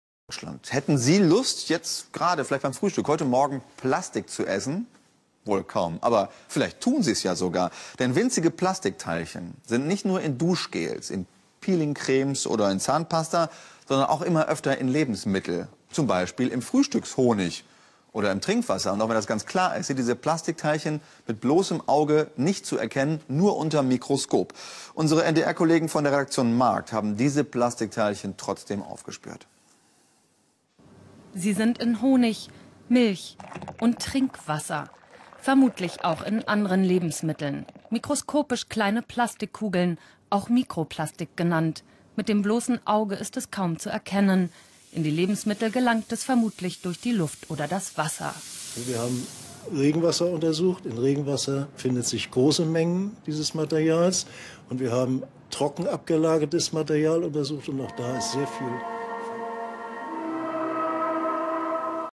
skywatchbretten - 27. November 2013 - Plastik im Himmel...
Das ist schon die vierte Woche mit Verdunkelung, kein Flugzeug ist zu hören. Gibt es mal ein bisschen blauen Himmel, dann wird sofort gesprüht.

""Wir können davon ausgehen, dass das Mikroplastik überall in der Atmosphäre zu finden ist", sagt Professor Gerd Liebezeit vom Institut für Chemie und Biologie des Meeres der Carl-von-Ossietzky-Universität Oldenburg. Die Vermutung: Das Plastik gelangt über die Luft auch in Lebensmittel. ""